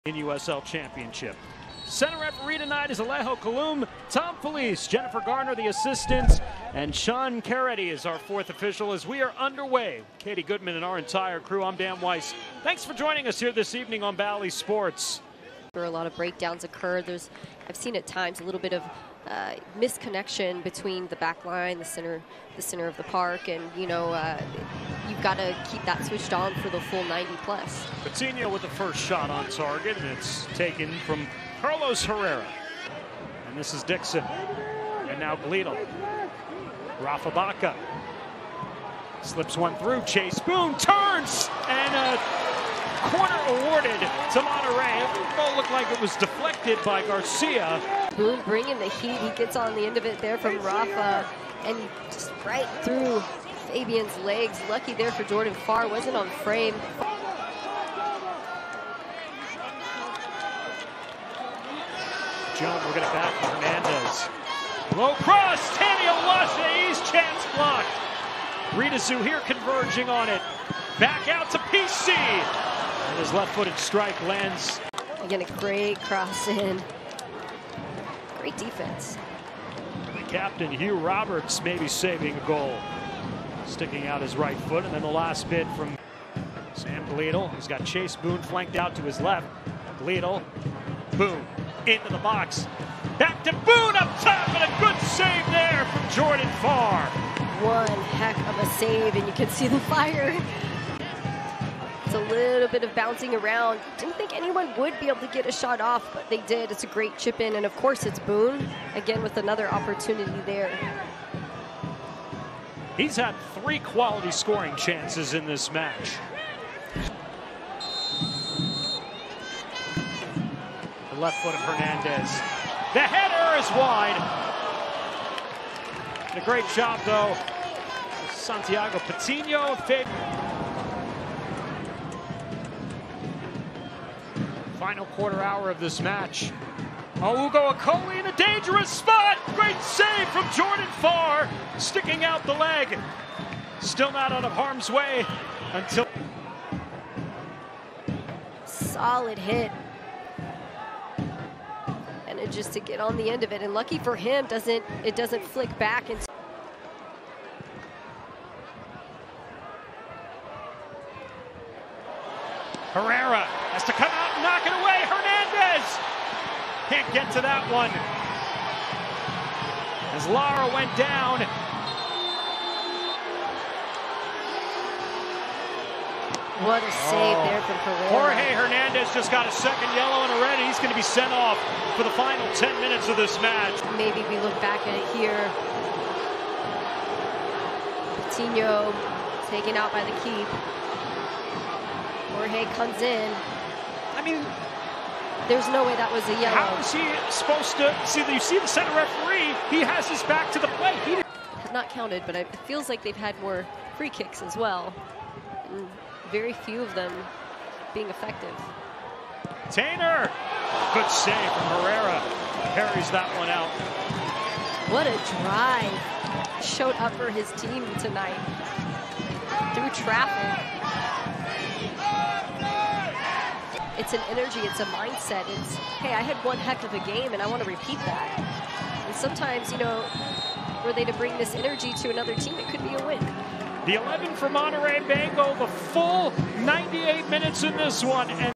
San Antonio FC vs. Monterey Bay F.C. - Game Highlights
Watch the Game Highlights from San Antonio FC vs. Monterey Bay F.C., 08/19/2023
Match Highlights presented by @Toyota!